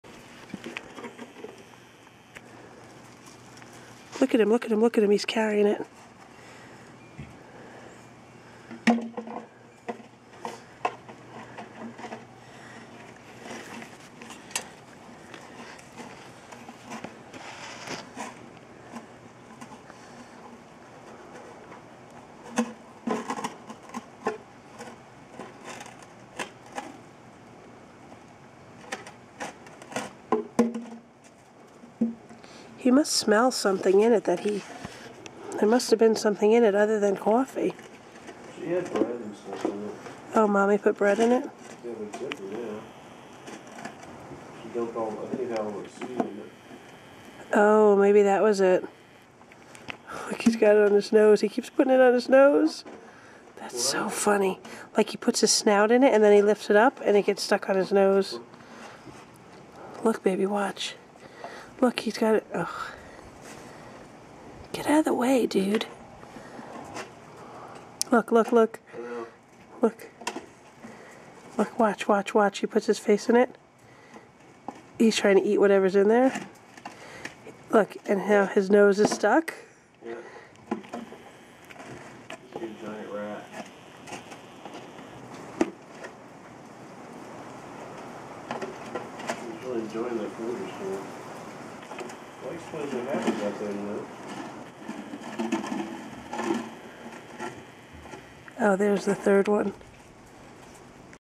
Javelinas and The Coffee Container
Javelinas late at night, foraging for food.  What they found was a coffee canister that they think they smell food in.